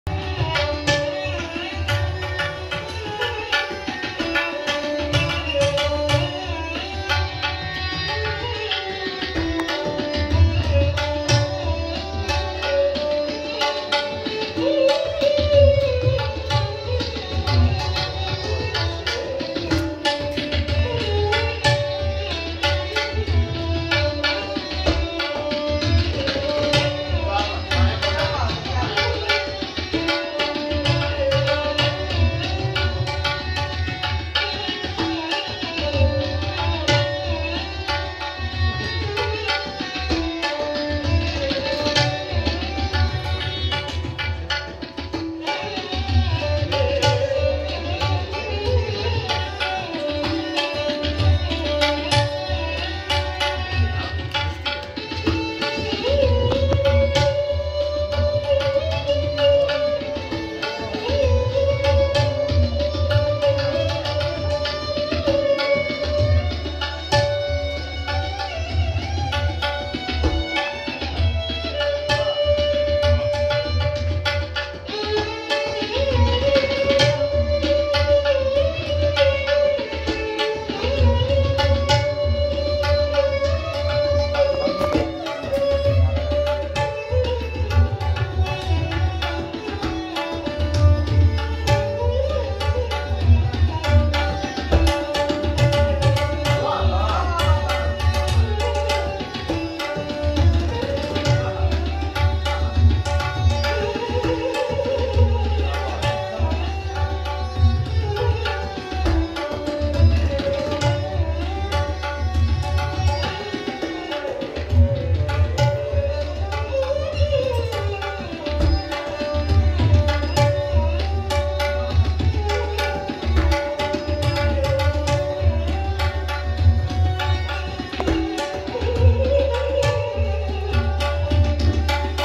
Soothing sound of Sarangi played by Taimur khan ❤
Taimor khan is a phenomenal Sarangi player who is into music and playing different instruments for more than 24 years. he started playing Guitar and then some other string instruments but finally he found himself eager to play Sarangi and select it as his main instrument to play. in his nun artistic life, he is a great doctor as well.